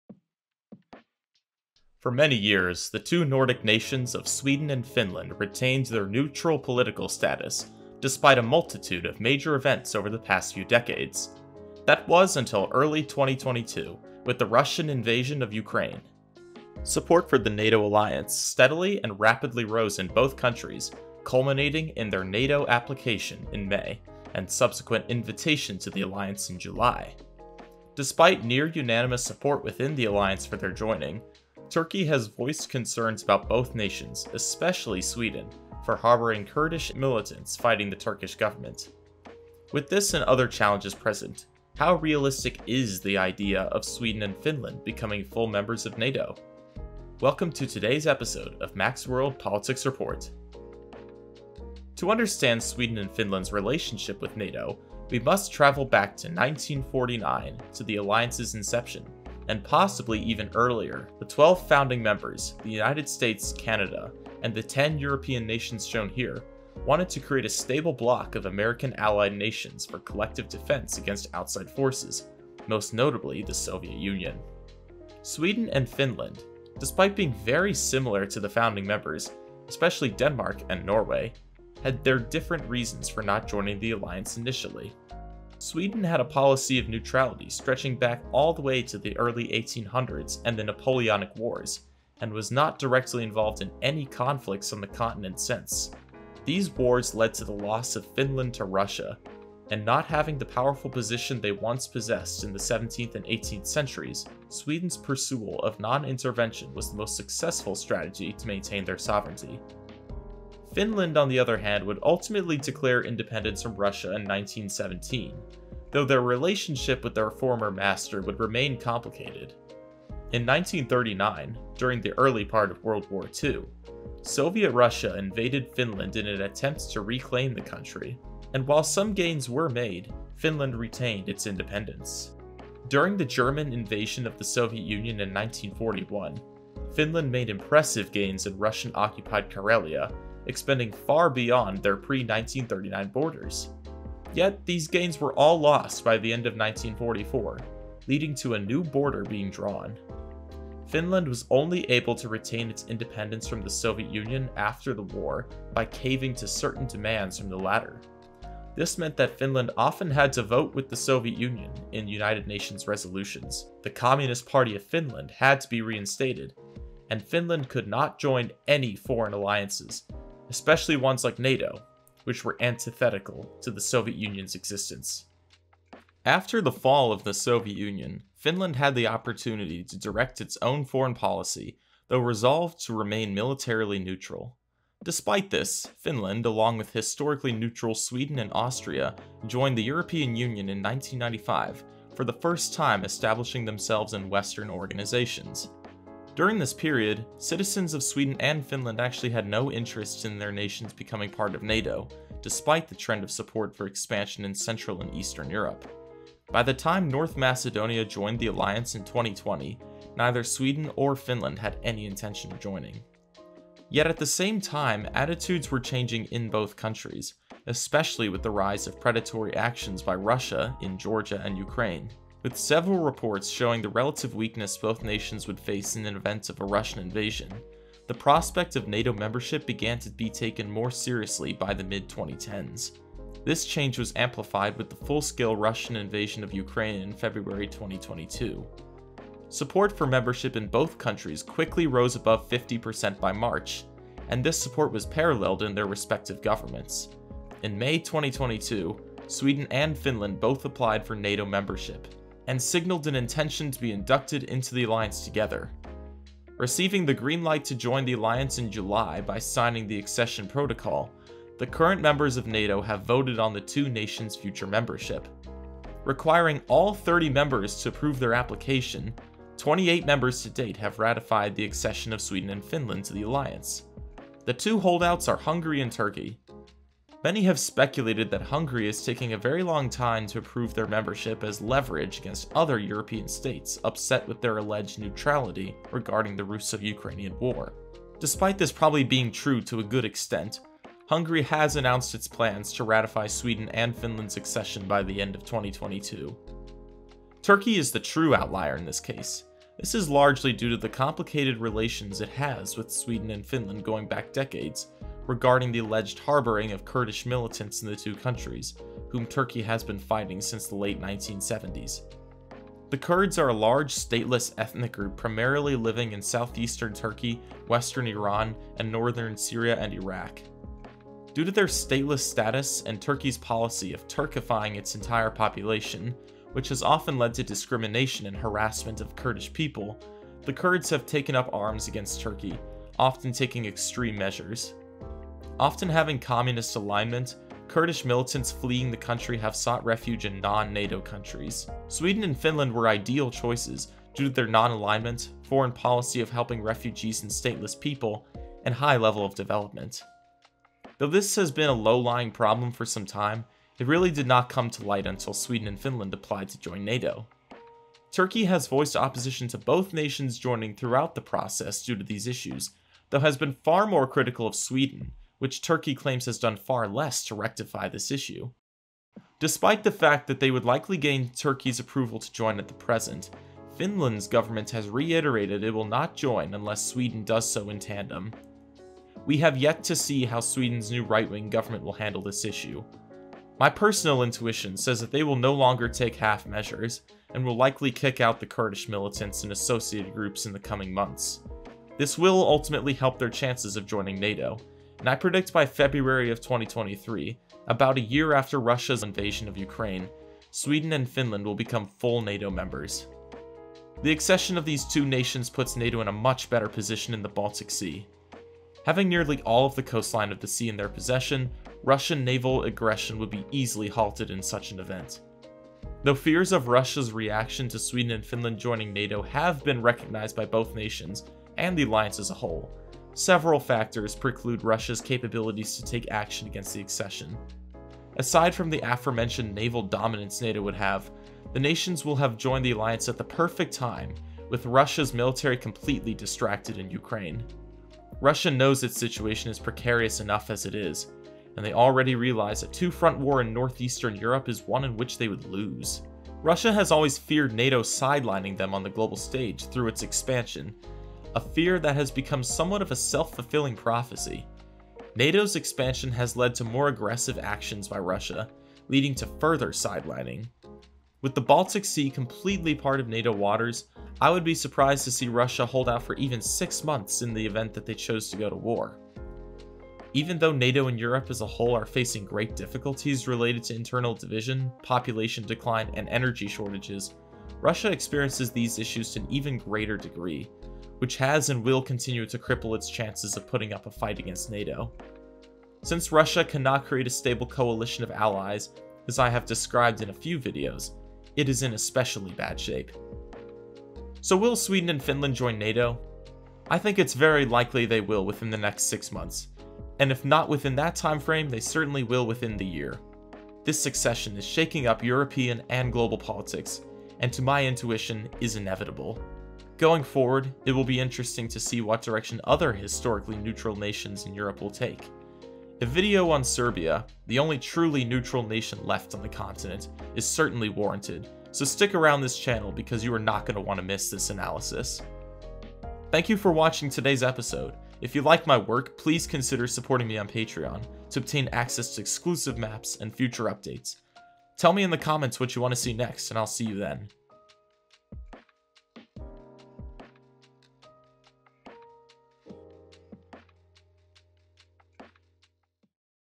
Can Sweden and Finland Still Join NATO?
Welcome back to another Mac's World political report. Today, I am covering the road Sweden and Finland have taken to join the NATO alliance, and what roadblocks remain in their way. I also discuss potential benefits for NATO to have these nations join.